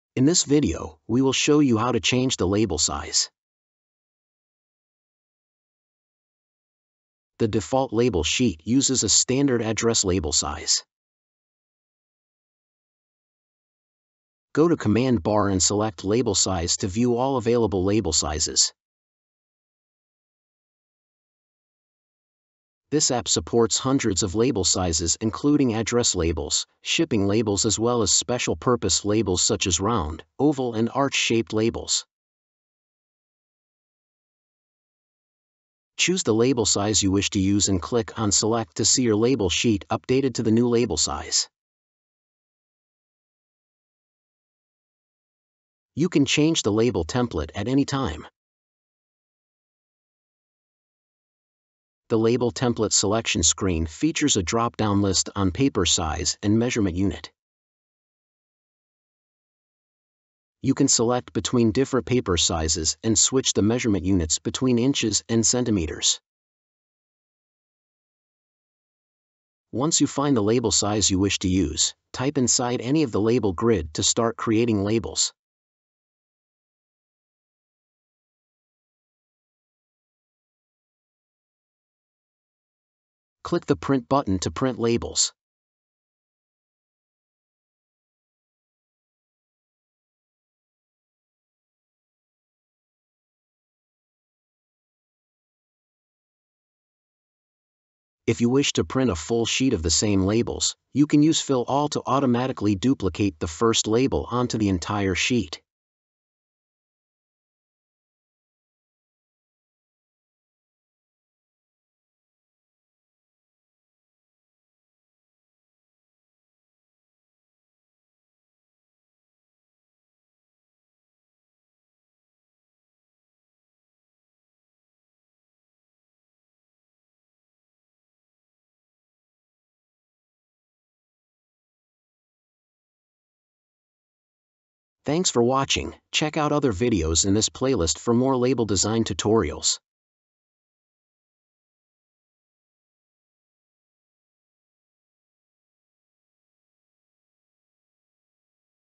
How to change label size | SimpleOne Label Designer for Windows 11 / Windows 10
In this video, we will show you how to change the label size.  The default label sheet uses a standard address label size.  Go to command bar and select "Label Size" to view all available label sizes.
SimpleOne Label Maker is available from the Microsoft Store.